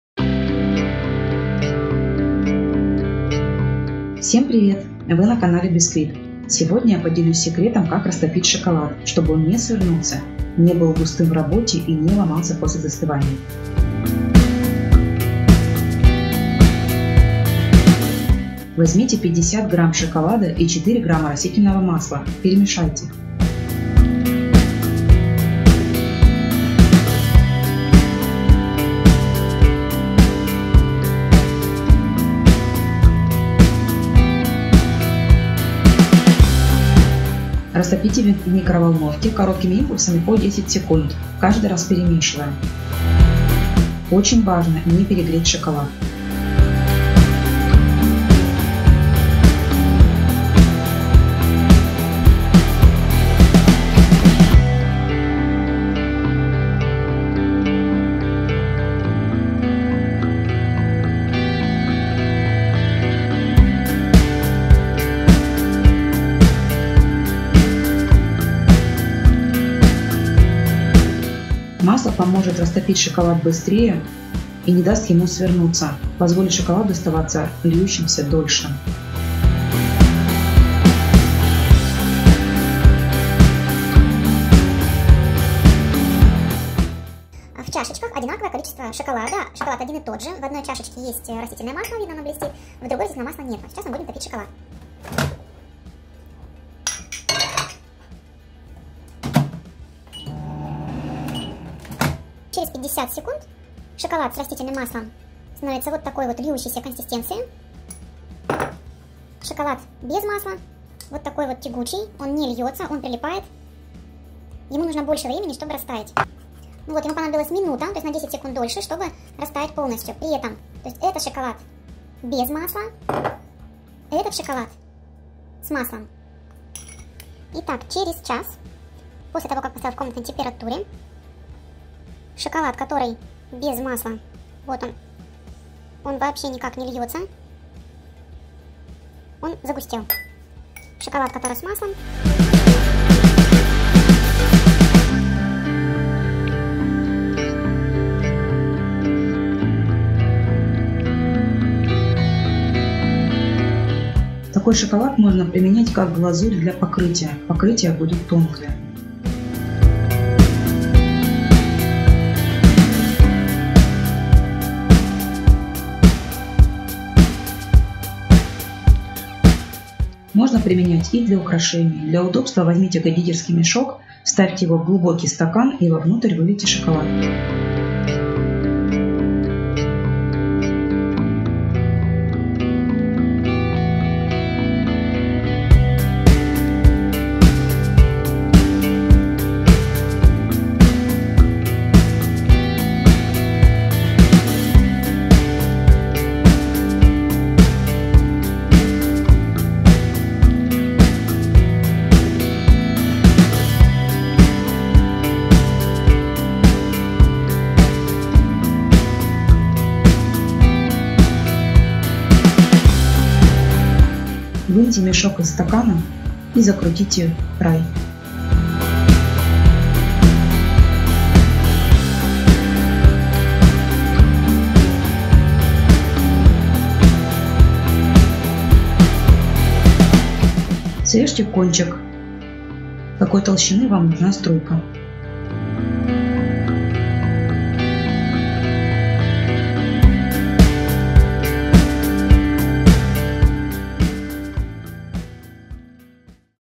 ГЛАЗУРЬ ИЗ ШОКОЛАДА. Как растопить шоколад для глазури | лайфхак
В этом видео вы узнаете: Как сделать вкусную, быструю и простую шоколадную глазурь? Как растопить шоколад, чтобы он не свернулся и не загустел при нагревании? Как сделать, чтобы шоколадная глазурь не ломалась при разрезании?

Ингредиенты:
Глазурь из темного шоколада:
темный шоколад - 50г/100г
масло растительное - 4г/9г

Глазурь из белого шоколада:
белый шоколад - 50г/100г
масло растительное - 5г/10г

Глазурь из молочного шоколада:
молочный шоколад - 50г/100г
масло растительное - 4г/9г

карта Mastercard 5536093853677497
Все средства пойдут на развитие канала. Спасибо!

VK (тут выкладываются не только видео-рецепты, но и интересные и полезные статьи)